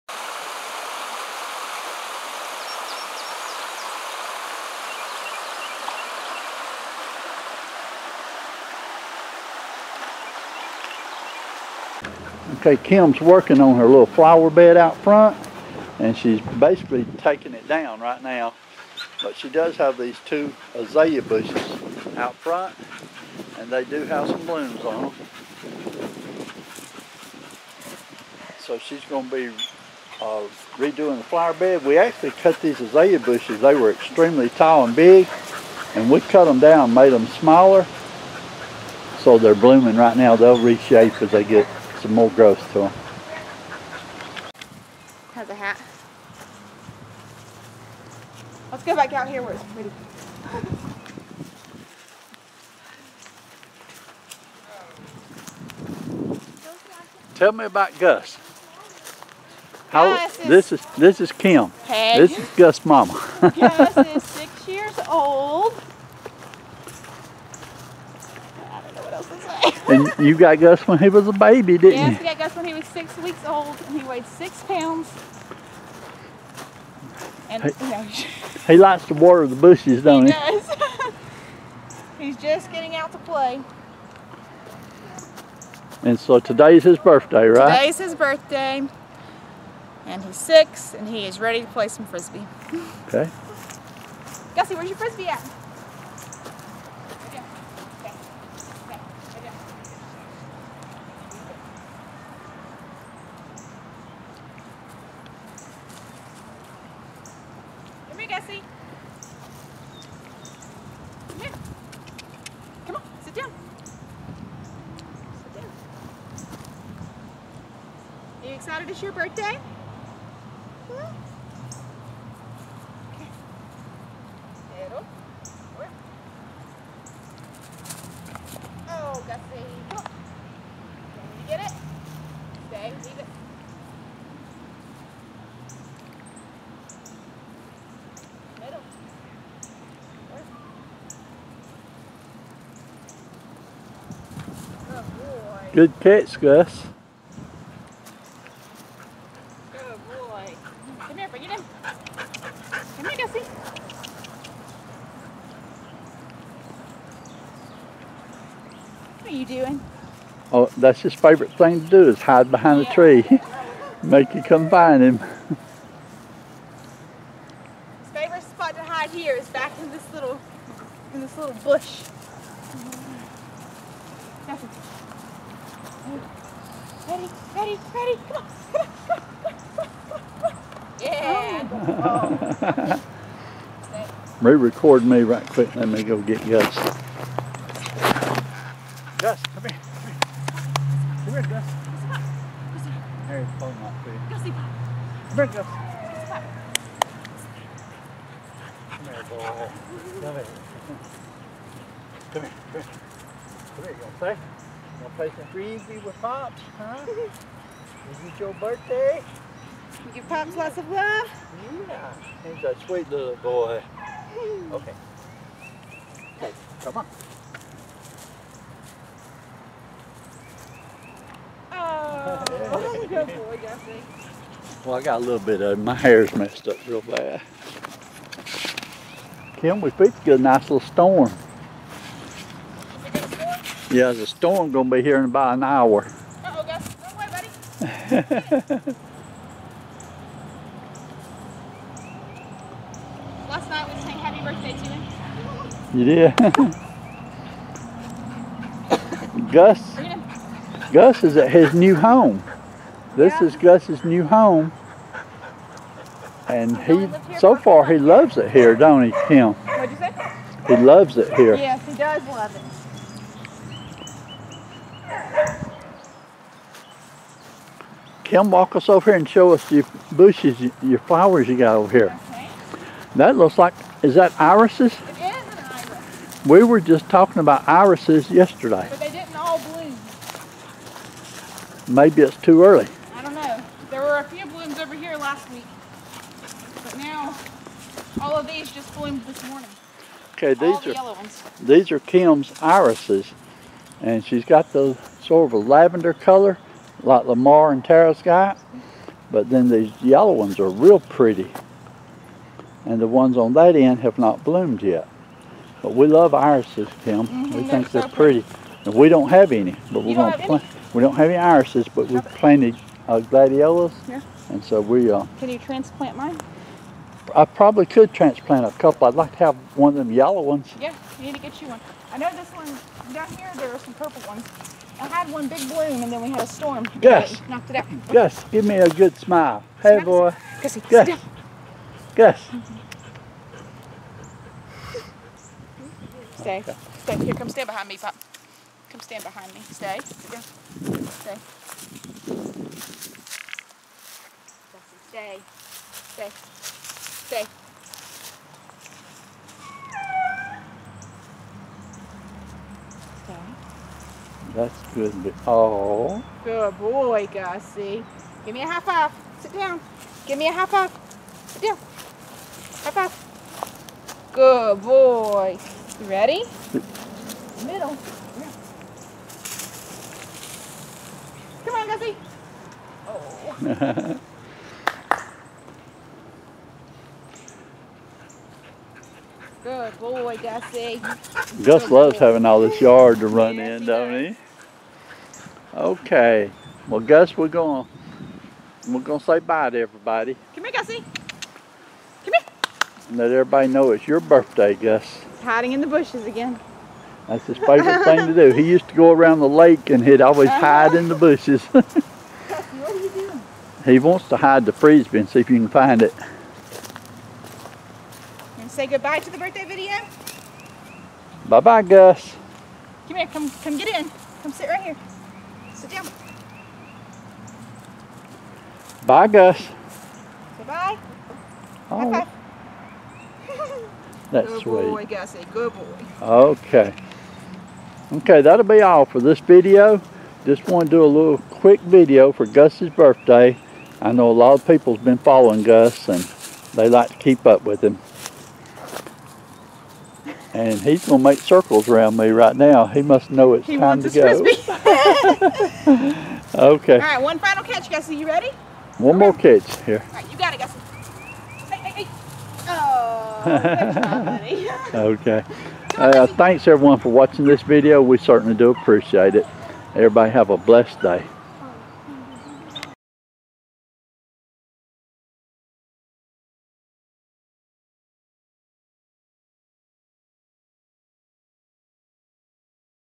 Happy Birthday Gus Six Years Old Border Collie
This is our granddaughter Kim's Border Collie.  His name is Gus and he is six years old today.